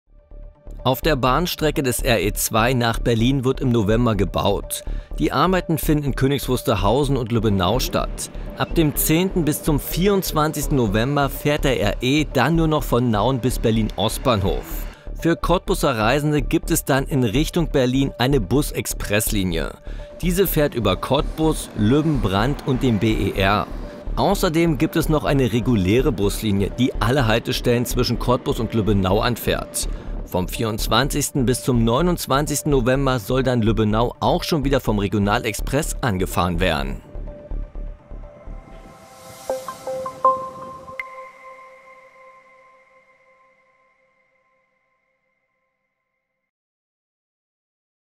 Zug um Zug, kommt man nicht nach Berlin
Die Strecke des RE2 nach Berlin. Viele Cottbuserinnen und Cottbuser sind jeden Tag auf ihr Unterwegs. Doch schon seit einiger Zeit geht da eher unregelmäßig alles nach Plan. Eine Baustelle nach der anderen und jetzt geht auf dem Gleis gar nichts mehr.